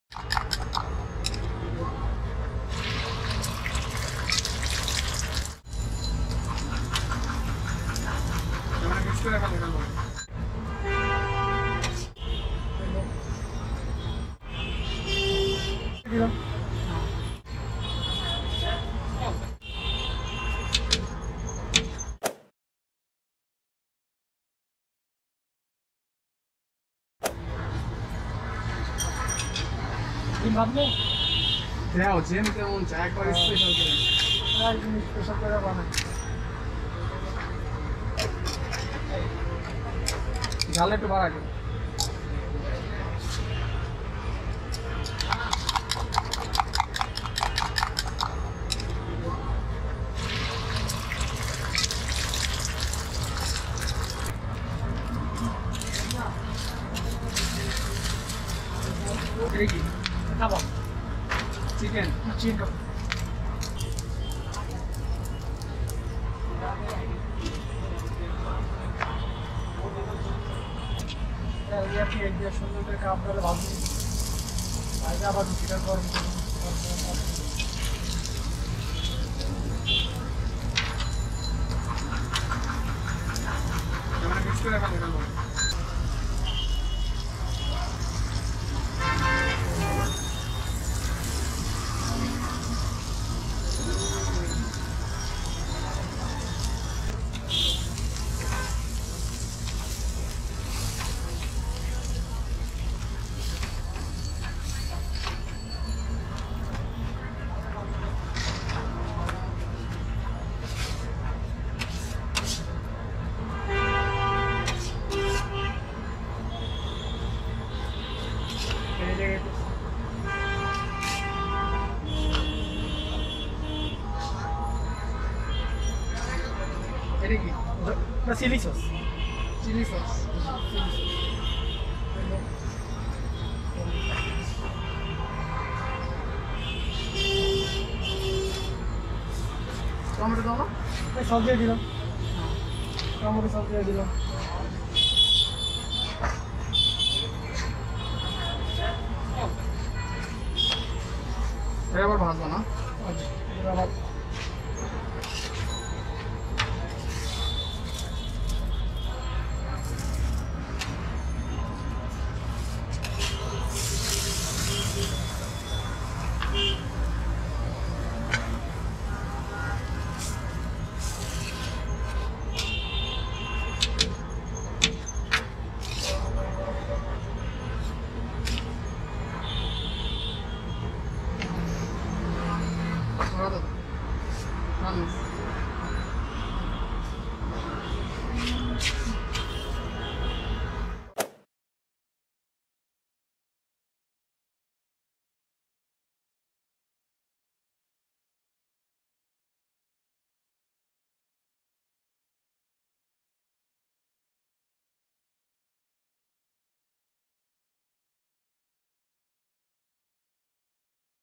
Tasty Chicken Tikka Egg Burger Recipe | Kabab Chicken Burger | Yummy Tasty Street Food
In This Video, You Are Watching The Most Popular Bangladeshi Street Food Tasty Chicken Tikka Egg Burger Recipe | Egg Burger | Kabab Chicken Burger | Yummy Tasty Street Food